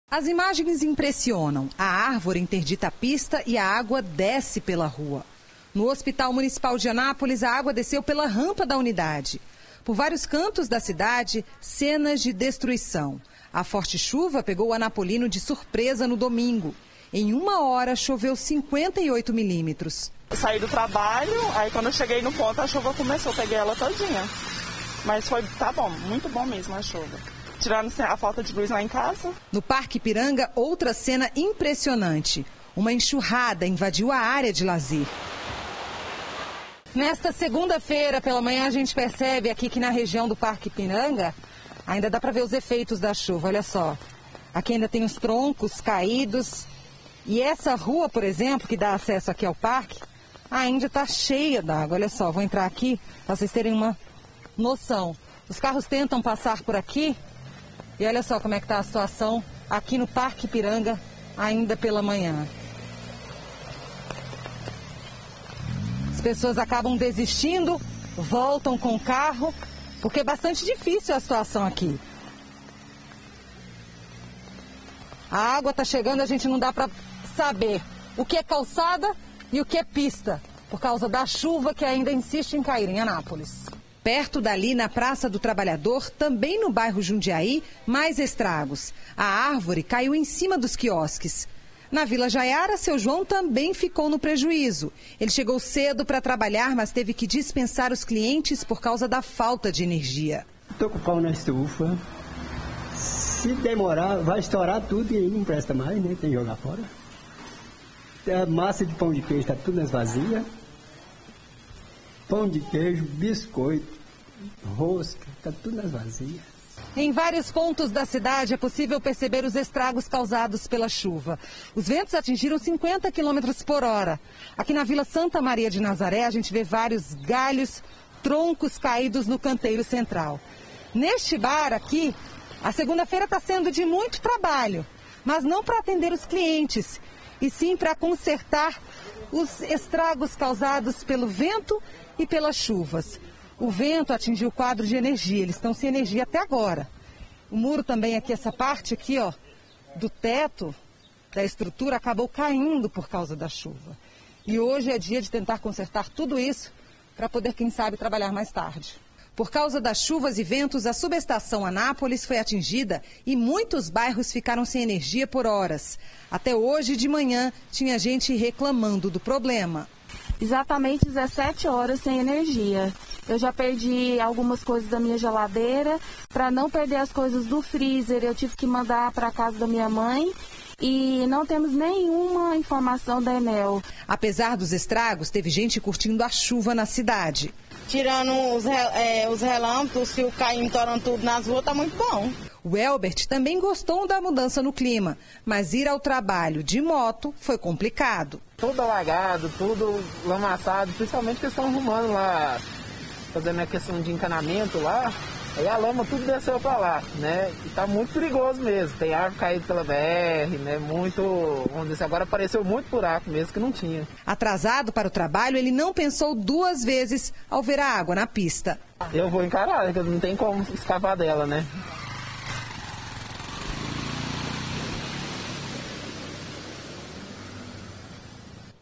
PREJUÍZO CHUVA E VENTANIA PROVOCAM ESTRAGOS
A CHUVA DE DOMINGO PEGOU MUITA GENTE DE SURPRESA, EM ANÁPOLIS. EM POUCAS HORAS, VARIAS PARTES DA CIDADE SOFRERAM COM INUNDAÇÕES, VENDAVAL E A QUEDA DE ENERGIA ELÉTRICA.